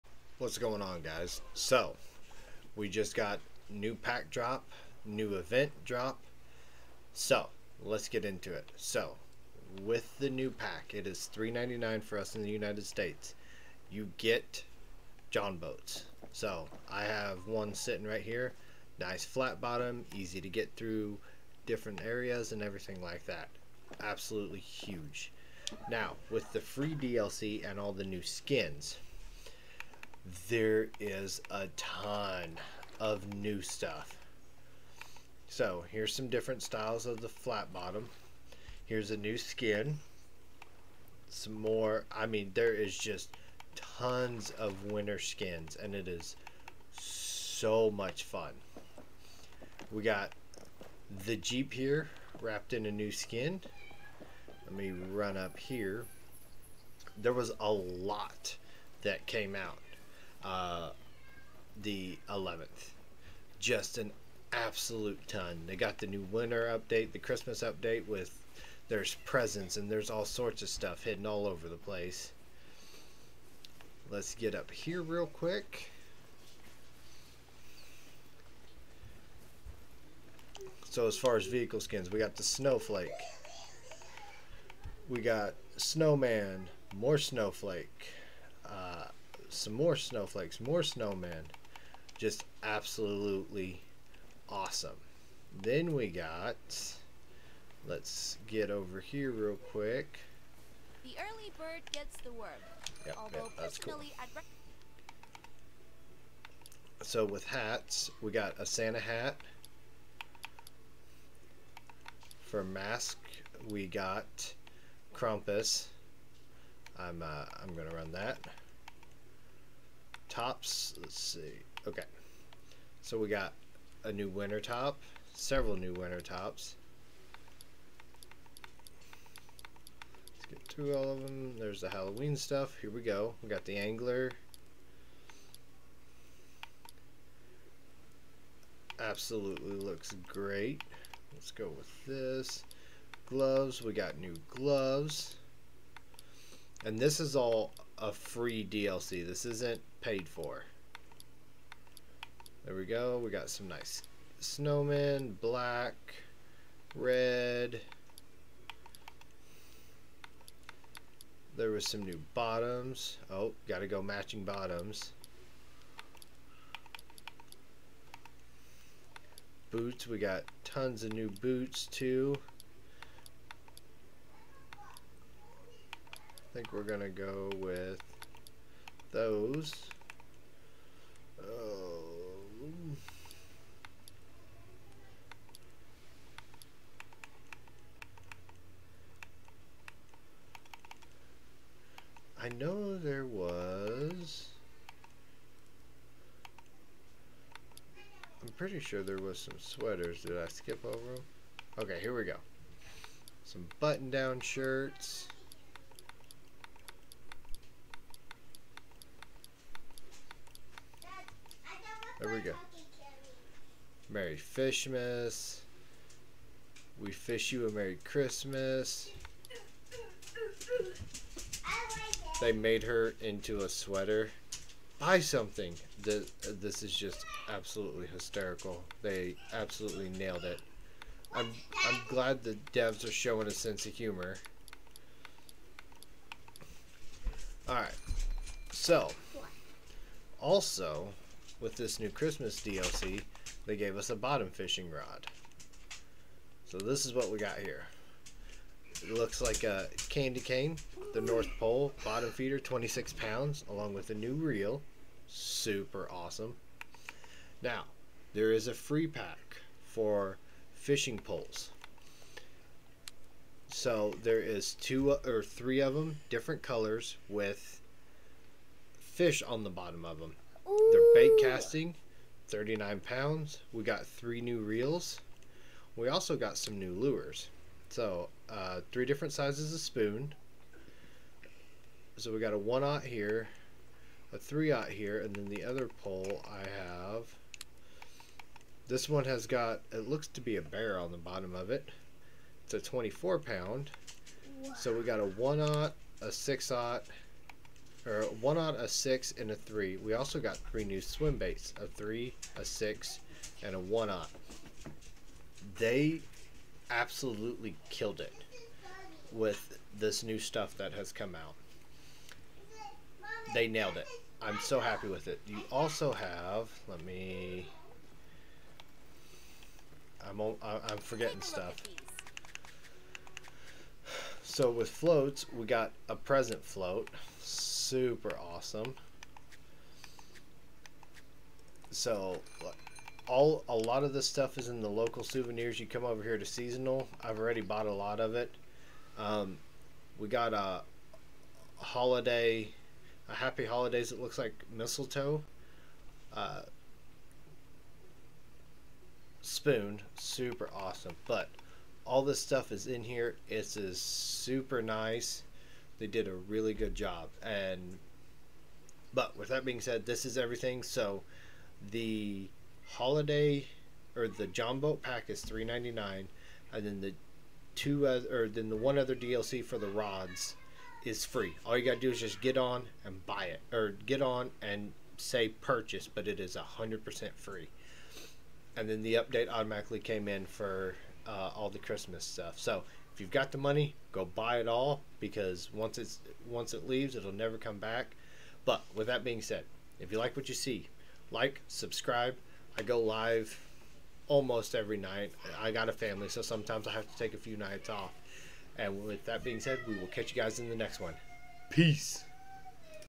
New dlc (flat bottom boats), new rod pack, and the winter update - 12/11/23
Going over the new stuff as of 12/11/23